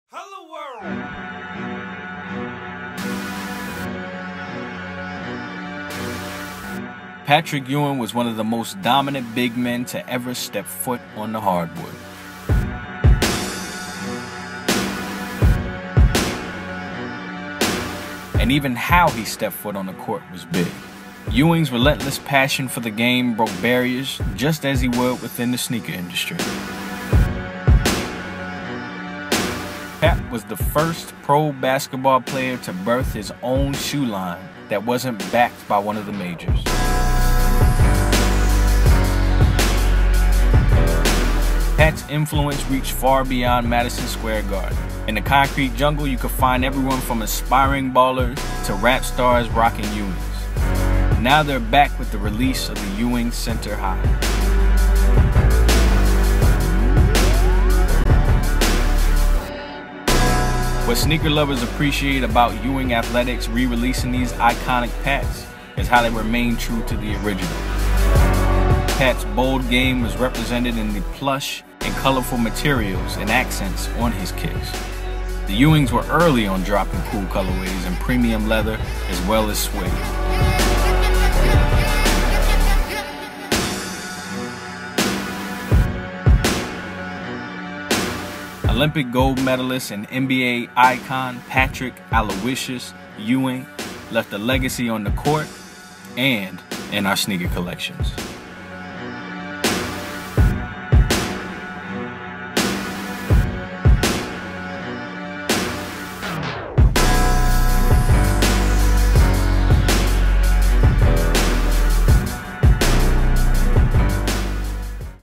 Art of MBB Episode 3: Patrick Ewing & The Ewing Center Hi
In this Episode, We take a look at the Legendary Patrick Ewing of the New York Knicks as well as Ewing Athletics and the Ewing Center Hi sneaker release. Directed by Esteban Serrano || Music by Dilemma.

You can also find more of my work and illustrating process on my website and other social media sites listed below:

Be Blessed